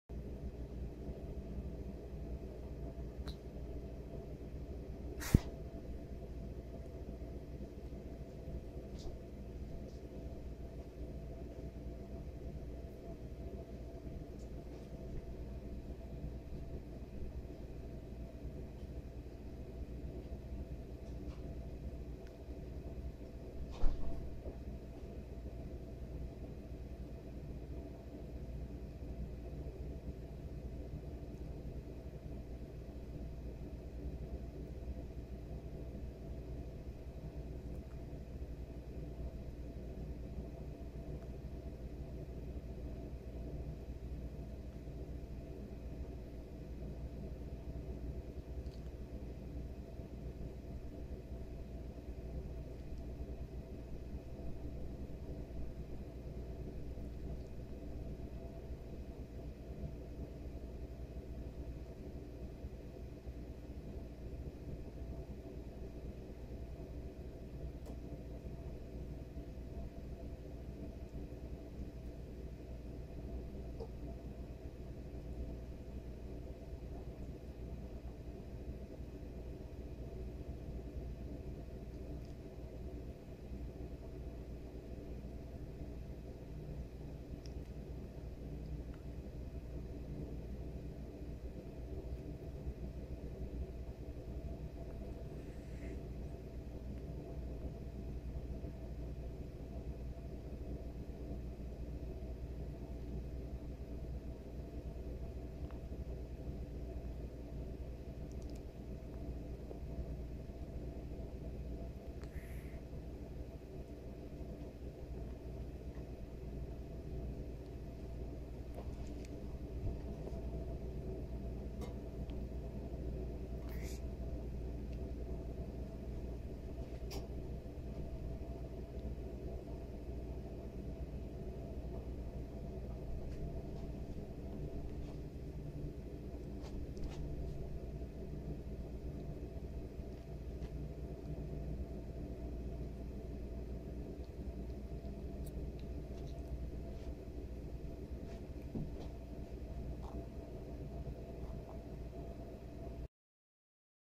Gila Eating
Gila Monster eating a mouse . We needed more room because he is a little close the camera . Recorded on a Nokia Lumia 1020 .